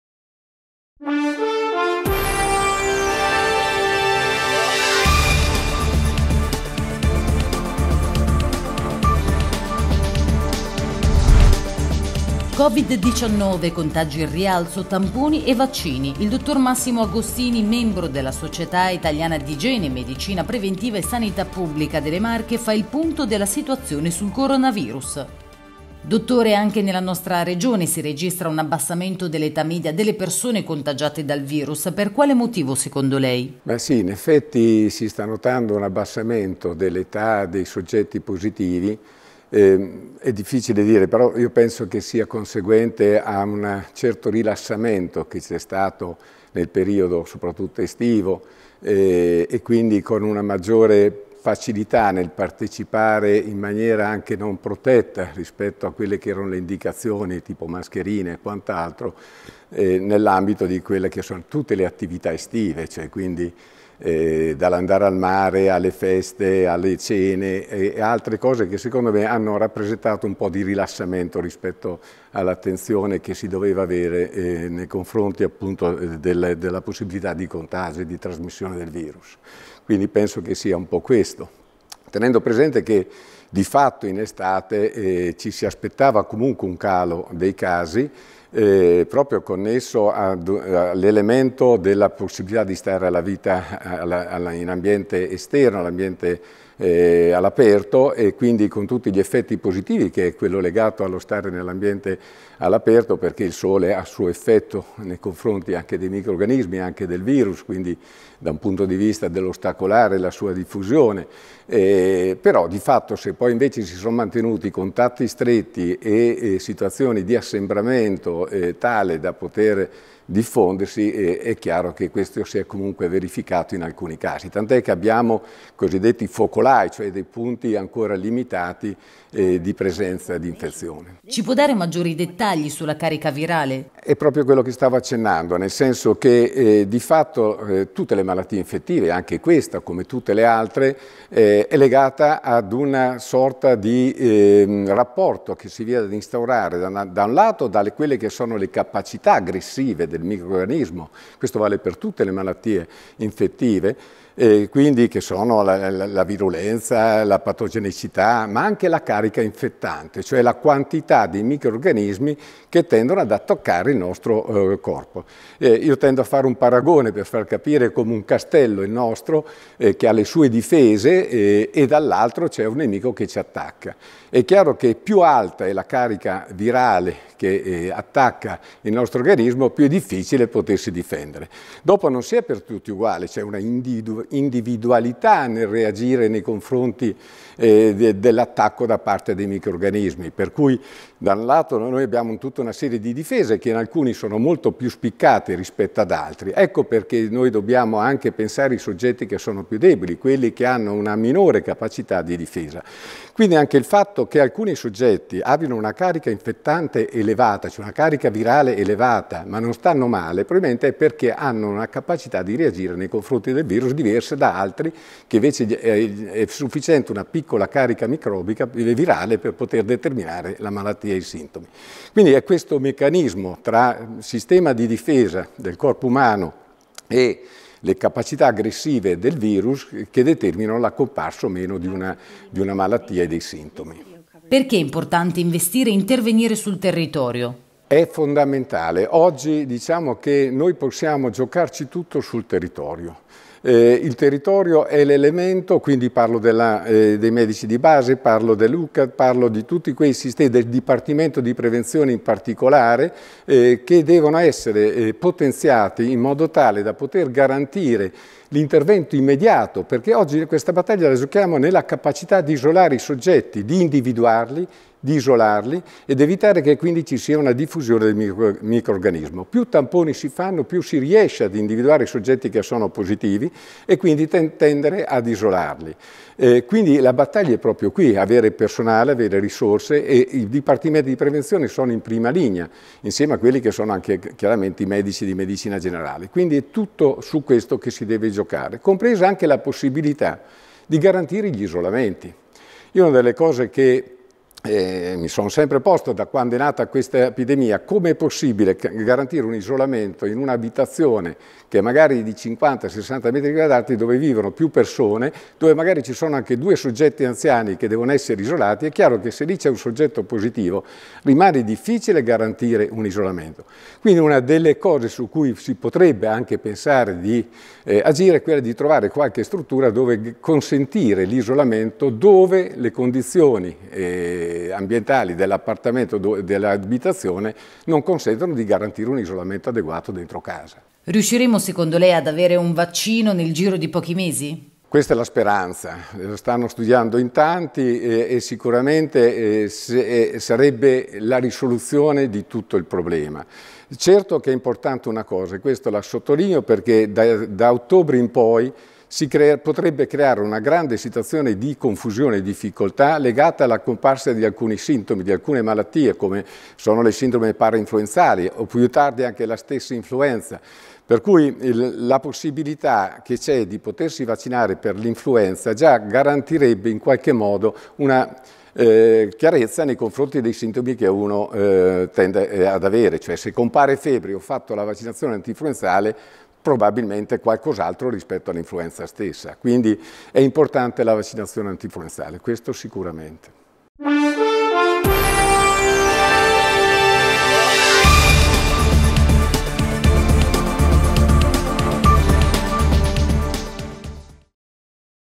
Contagi e vaccini, parla l'esperto - Approfondimento
Coronavirus. Migliorano le condizioni cliniche del paziente 60enne di Fano ricoverato in terapia intensiva dopo aver contratto il Covid. Nei giorni scorsi era stato trattato con il plasma iperimmune abbinato a un farmaco. Ora non è più intubato e respira autonomamente. Intanto nelle Marche venerdì si registrano 17 tamponi positivi, di cui 6 nella provincia di Pesaro e Urbino. Questi casi riguardano rientri dall’estero, ma anche persone contagiate durante la movida. Di Covid-19, servizi sanitari e vaccini ne abbiamo parlato con il dott. Massimo Agostini, membro della Società Italiana di Igiene e Medicina Preventiva delle Marche.